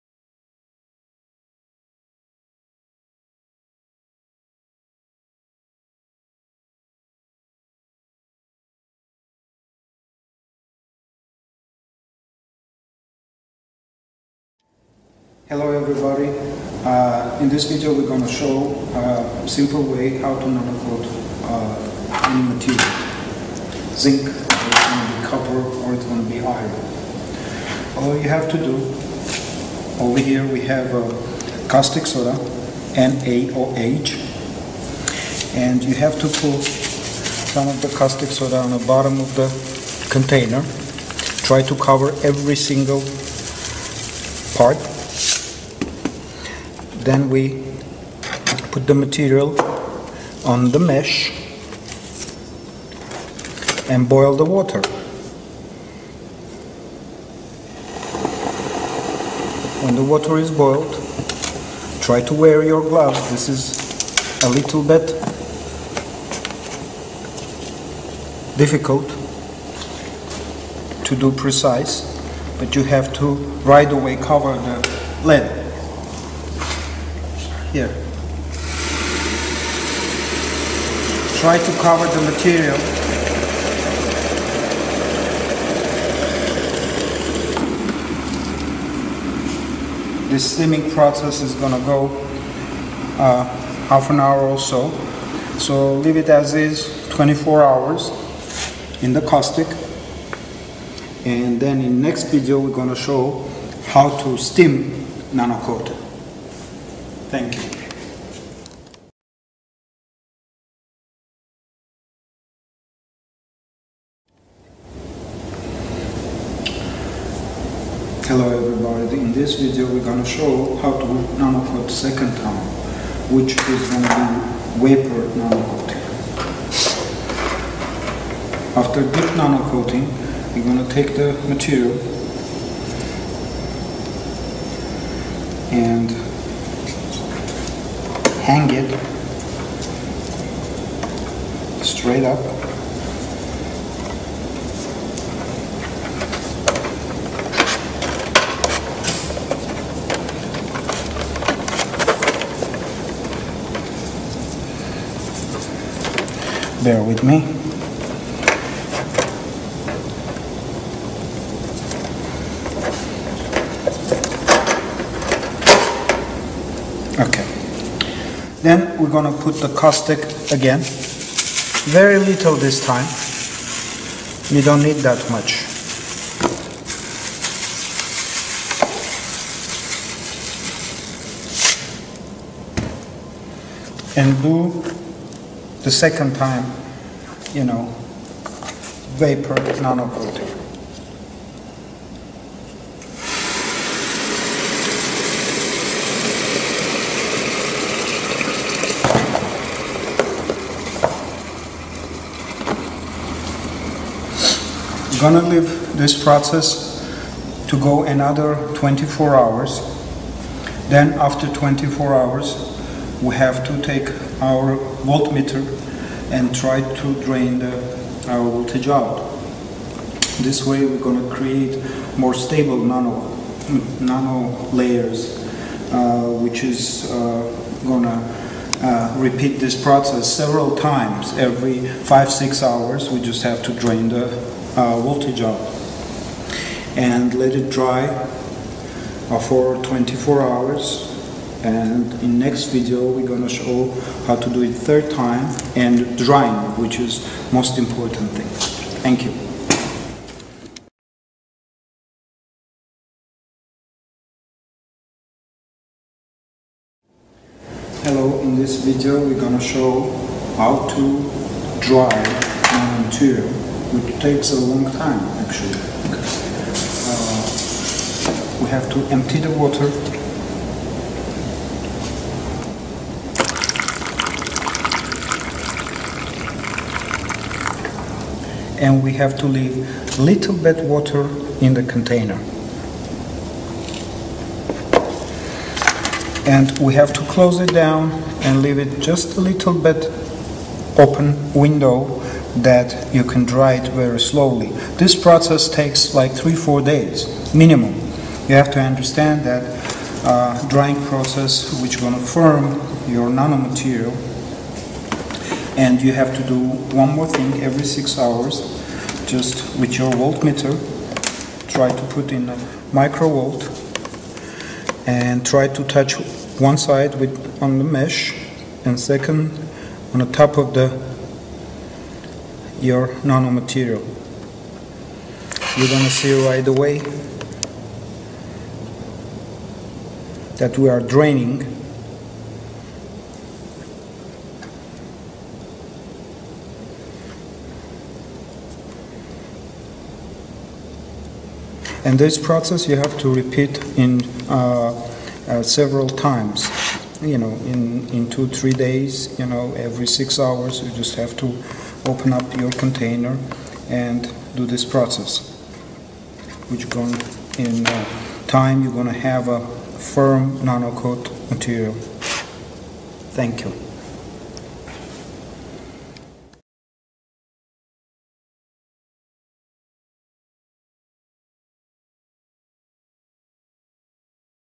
Acoperirea Nano - Tehnologia Keshe
Tutorial despre acoperirea materialelor cu straturi nano, facut de Arman de la Institutul Spatial Keshe din Italia.